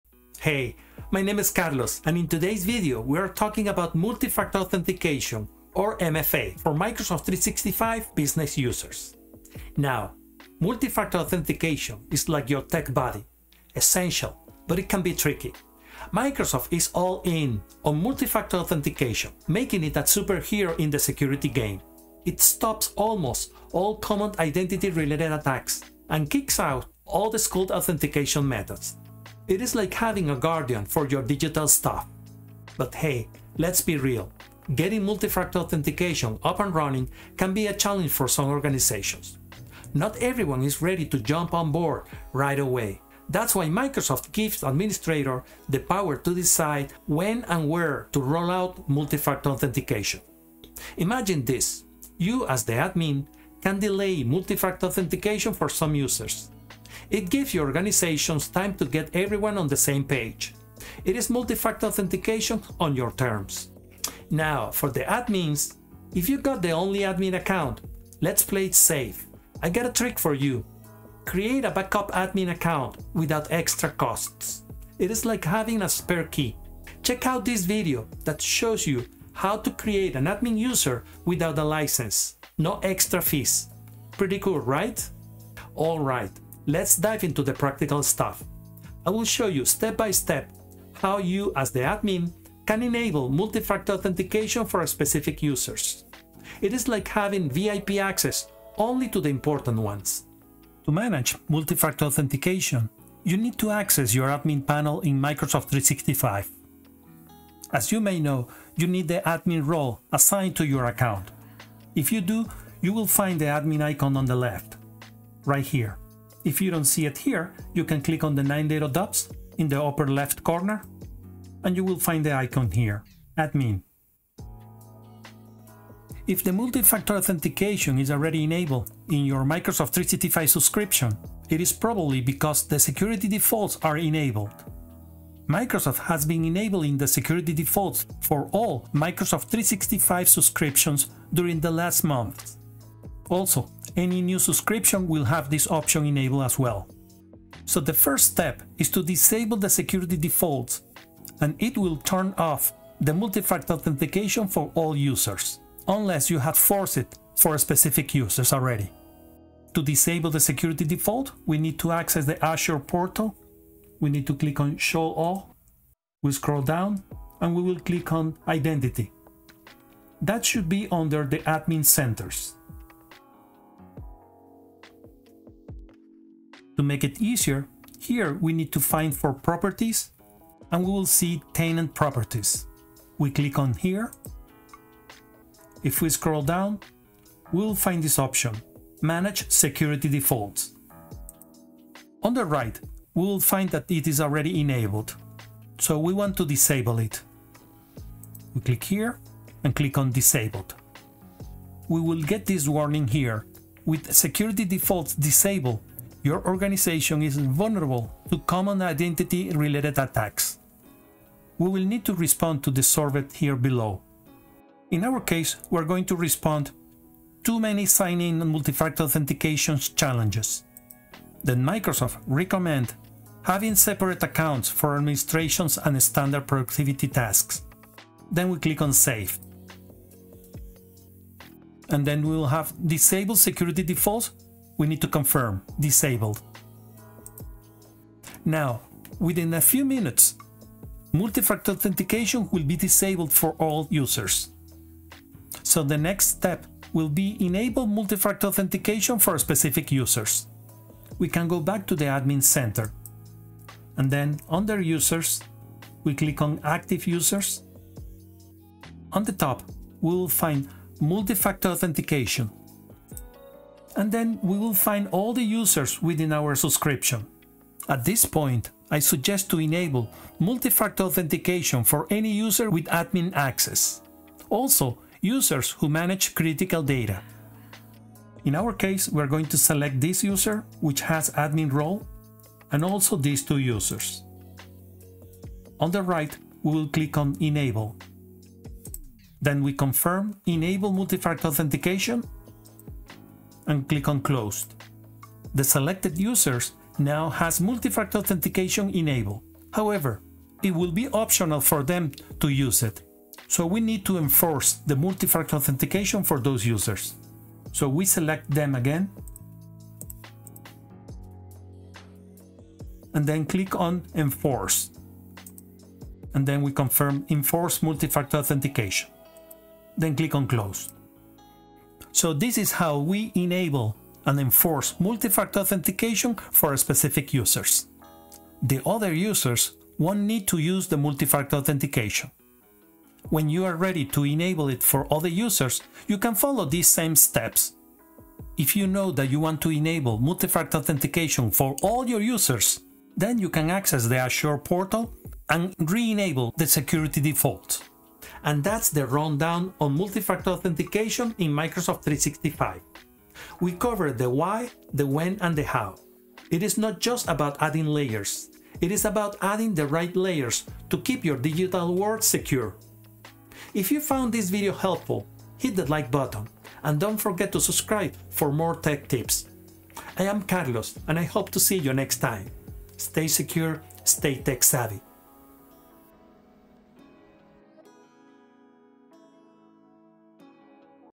Managing Multi-Factor Authentication in Microsoft 365 for Business
You will learn how to Manage Multi-Factor Authentication and enable it for selected users in your Microsoft 365 Business Subscription.

You will learn how to disable the Security Defaults (to disable Multi-Factor Authentication for all users) and then re-enable MFA for specific users in Microsoft 365 Business.

*** PLANNING TO DEPLOY AND MIGRATE TO MICROSOFT 365? ***

00:00 Introduction
00:33 The MFA challenges
00:51 Strategic MFA Rollout
01:04 Admin Tip - How to create an admin user without a license
01:25 Selective MFA Activation Demo
02:05 How to disable Security Defaults
04:06 Enable MFA for selected users
05:03 Enforce MFA for selected users
05:47 Conclusion